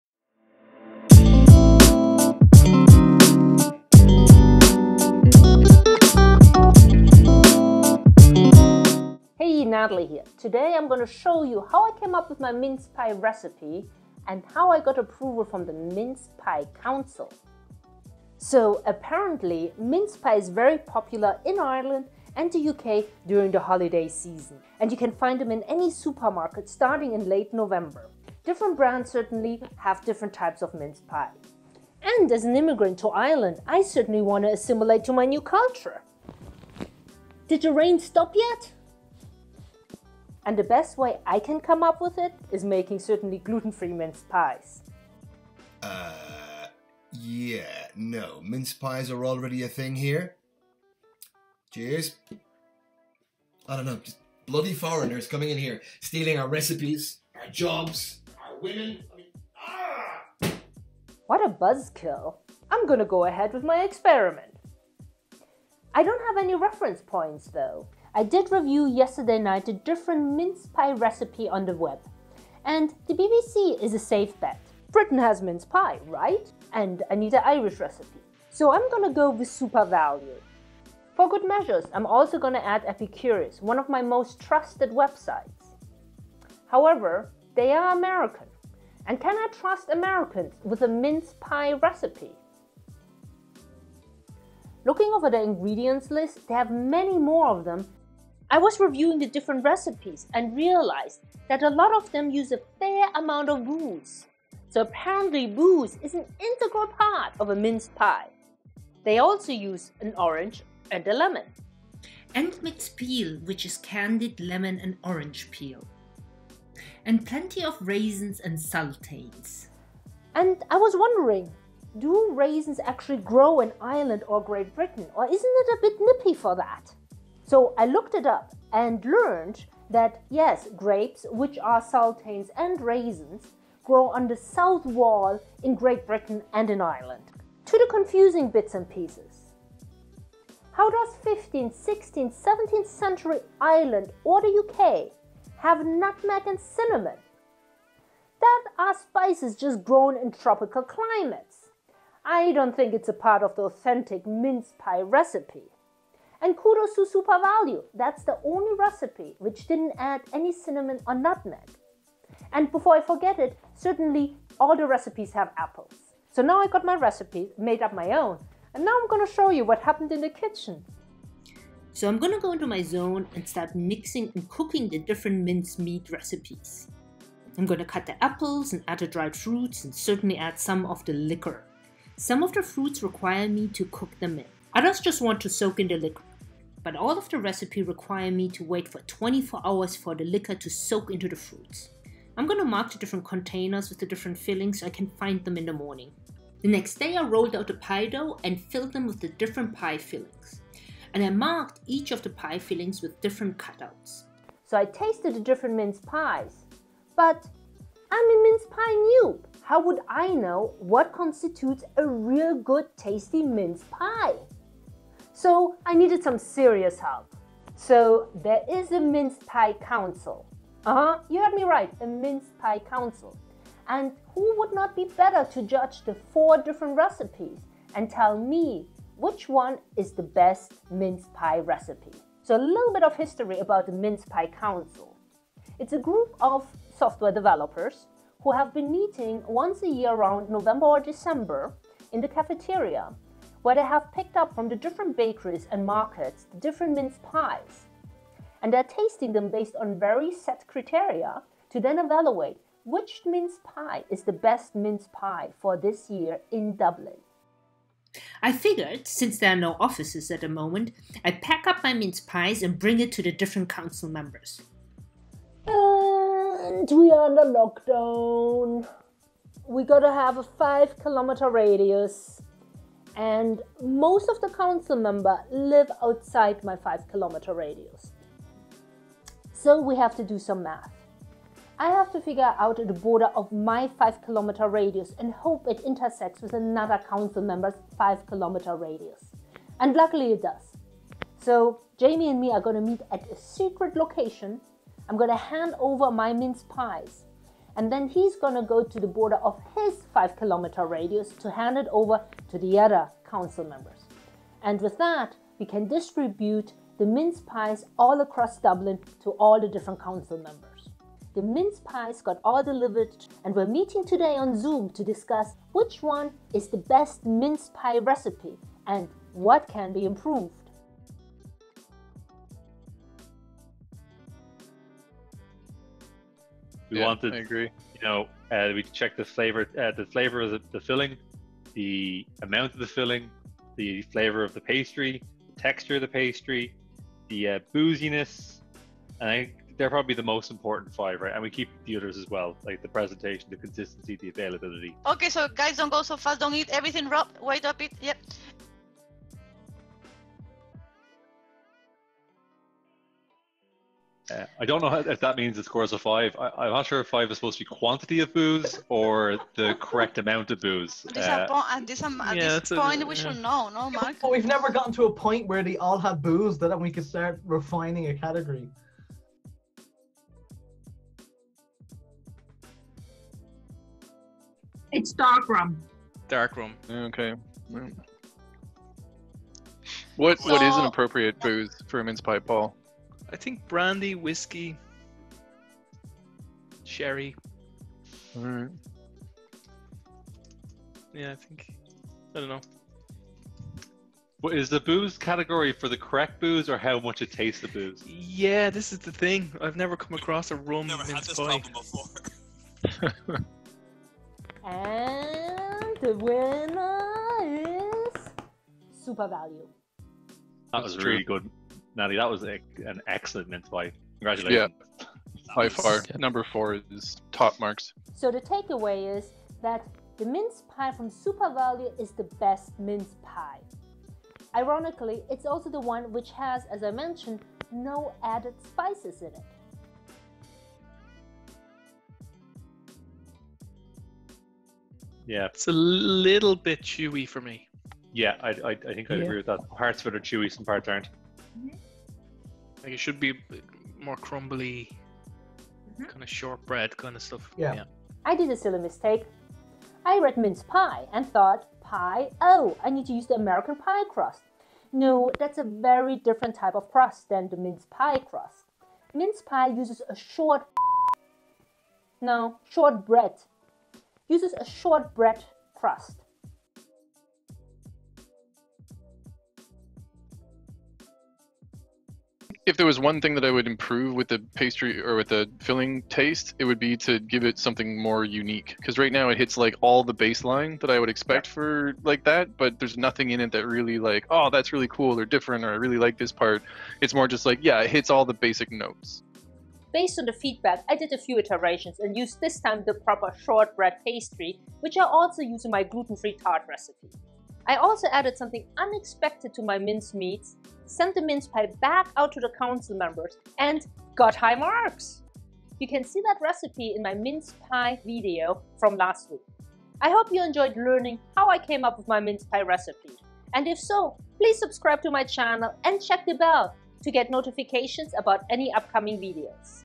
What is the BEST Mince Pie Recipe? Gluten Free.
We are reviewing this week with the Mince Pie Council four different MincePie recipes from SuperValue, Epicurious, BBC, and my own mixture of MinceMeat. Get the insights of what the Mince Pie Council was looking for in their evaluation of the different recipes and which one is the winner. The revised (and oh-so-delicious) winner recipe was featured in last week's Mince Pie Recipe.

Special Thanks go this week to Dermot Magennis for his acting and lovely voice in the intro, and all the Mince Pie Council Members.

Chapters

0:00-1:07 Intro
1:08 - 2:50 Selecting and Comparing 3 Mince Pie Recipe
2:51- 3:23 Glimpses on Making the Recipes
3:24 - 3:50 How to compare Mince Pies?
3:52 - 4:12 Intro to the Mince Pie Council
4:13 -5:10  How to transport Mince Pie During Lockdown?
5:11 - 7:09 The Council Meeting and the Winner Is.
7:10 - 8:39 How to Improve the Mince Pie Recipe